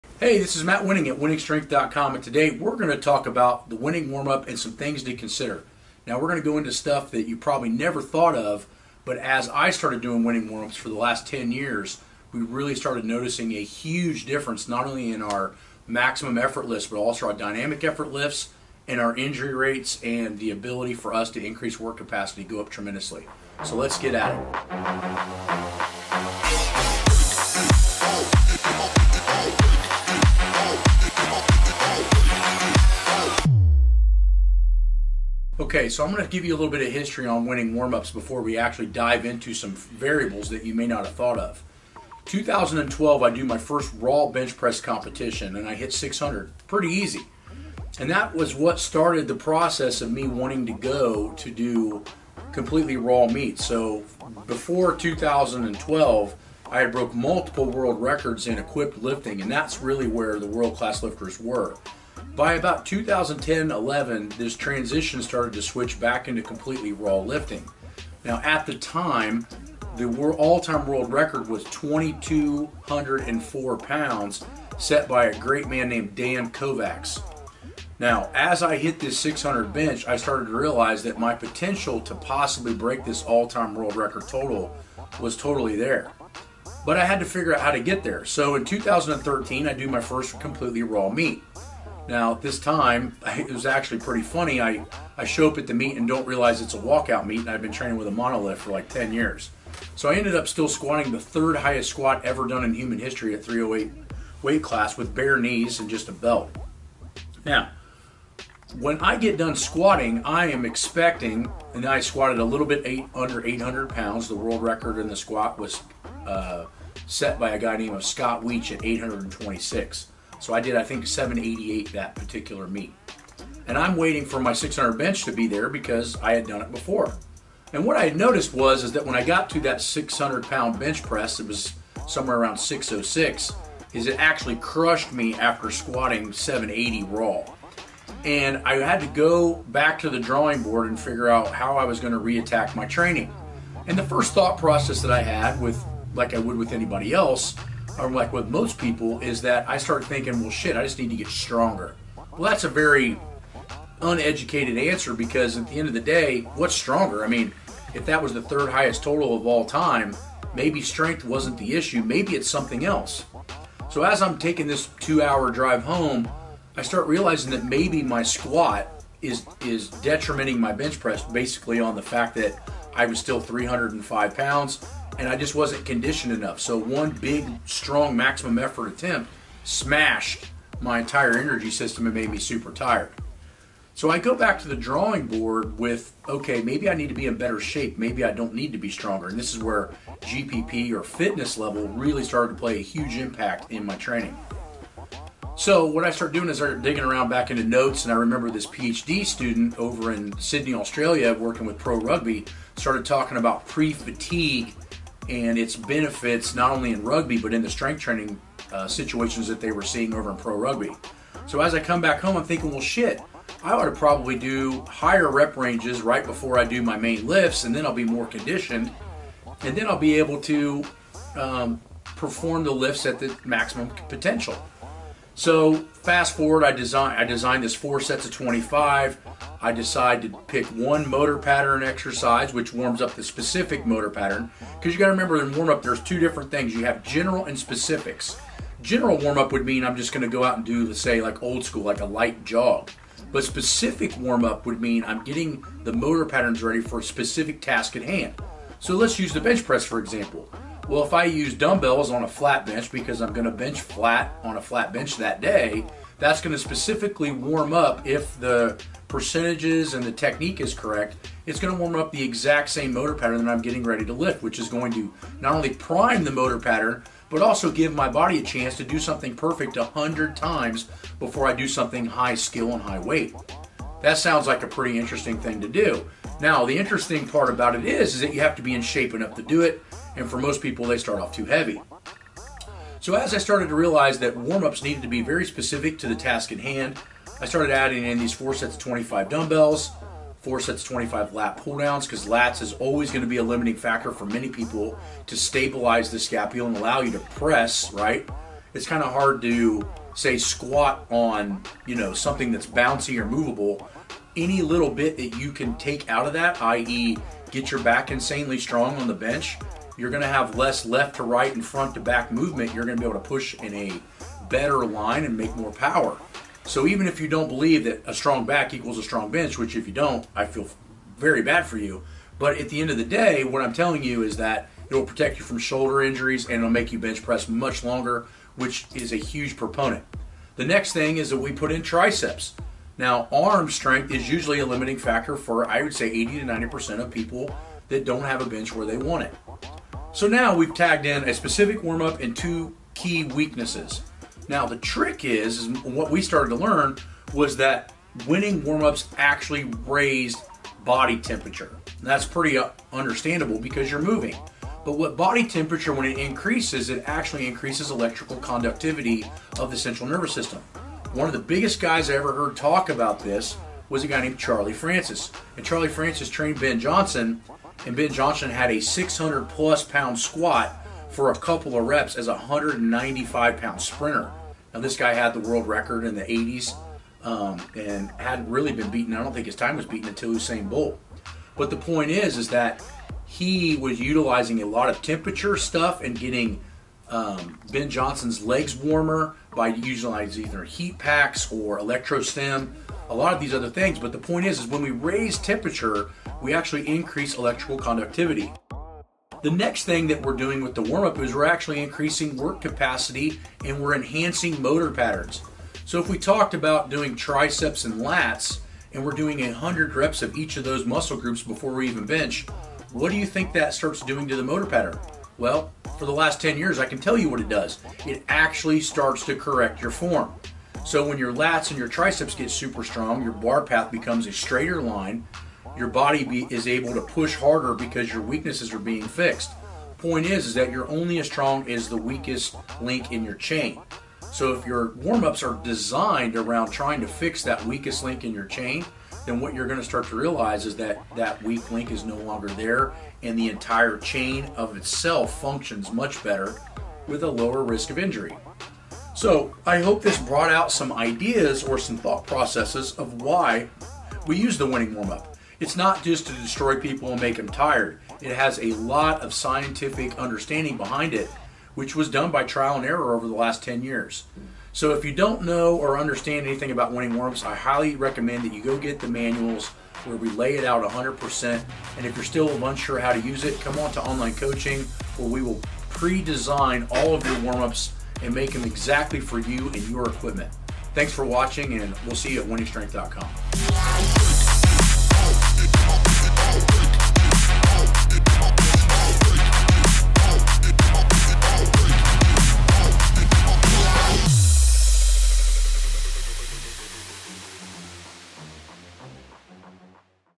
The Science Of Wenning Warm-Ups (The Best Science-Based Warm-Up Routine)
Matt discusses the science and theory behind Wenning Warm-Ups. Wenning Warm-Ups are a Ludus Magnus staple, and in this video, Matt breaks down the hows and whys of Wenning Warm-Ups.